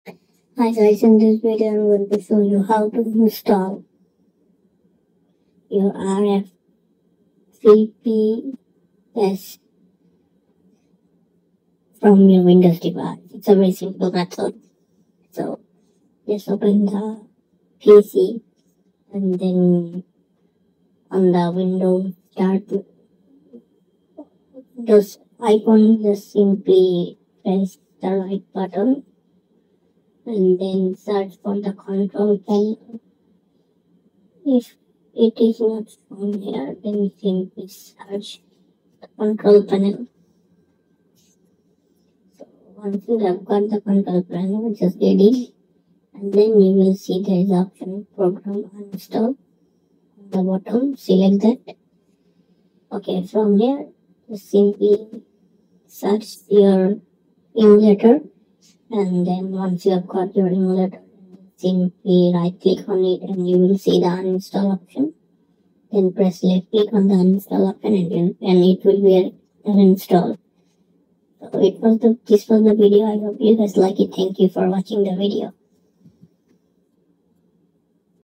How to Uninstall RPCS3 Emulator on PC (2025)
How to Completely Uninstall any Emulator from PC

In this video, I'll show you How to completely Uninstall Emulator from your Computer . The method is very simple and clearly described in the video. Follow all of the steps & How to Uninstall RPCS3 Emulator on PC